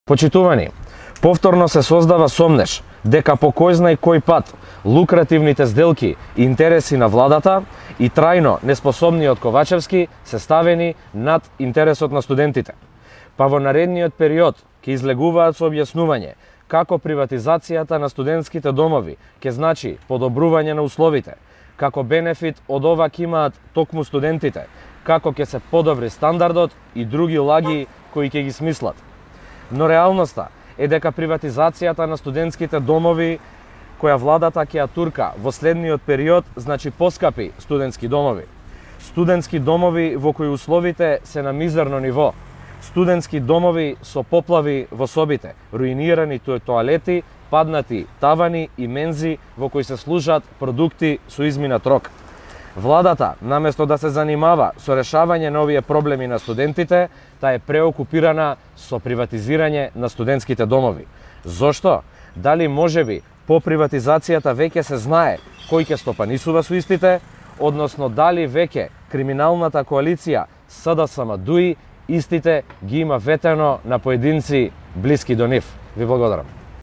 Попов: Владата наместо да се занимава со решавање на проблемите на студентите...
Попов: Владата наместо да се занимава со решавање на проблемите на студентите, се занимава со приватизација на студентските домови

Изјава за медиуми на Сергеј Попов, преседател на УМС на ВМРО-ДПМНЕ,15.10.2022